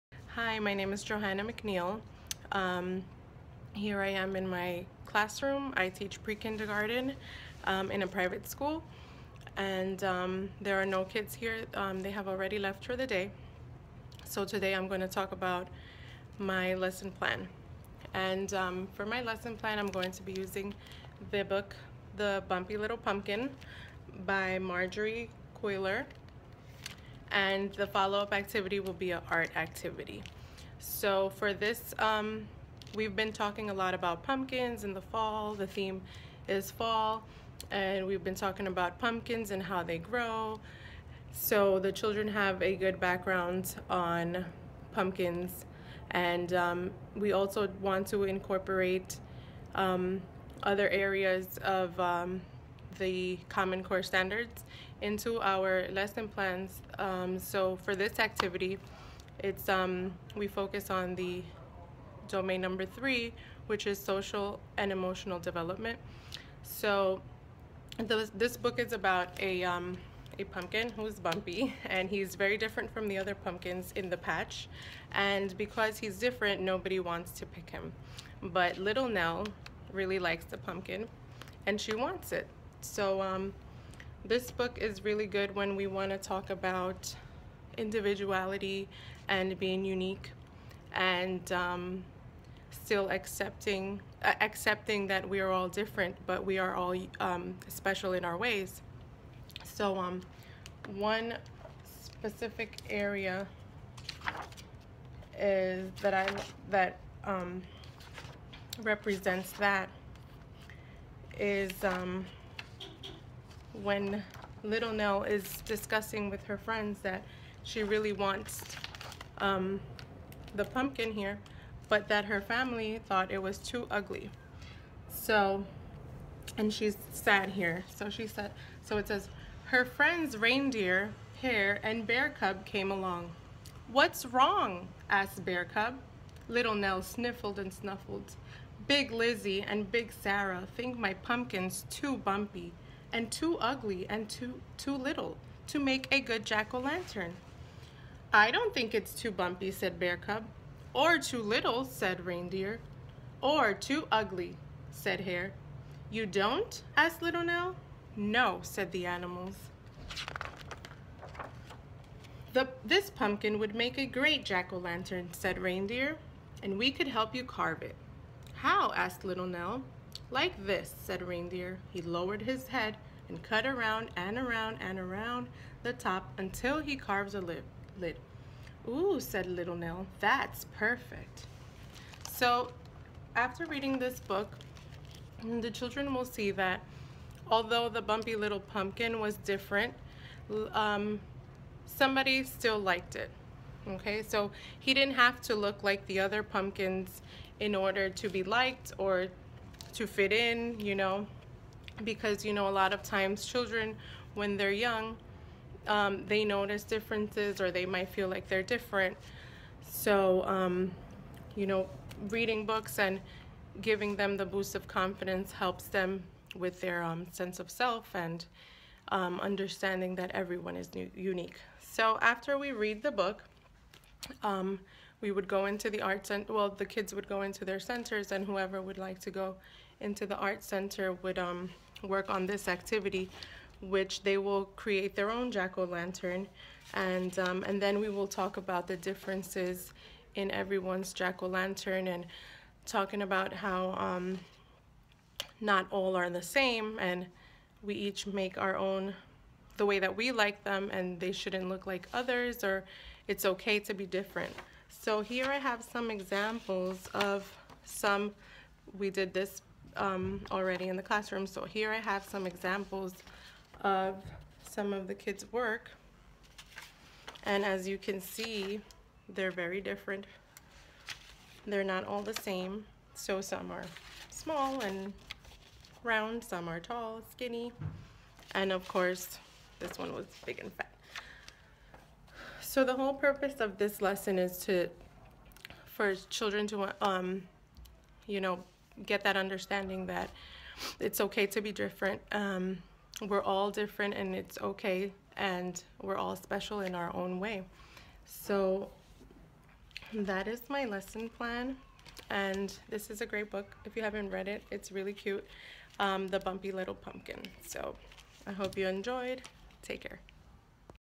Simulated Sample Lesson Plan
This video was made as a course requirement in my graduate program in preparation for the EdTpa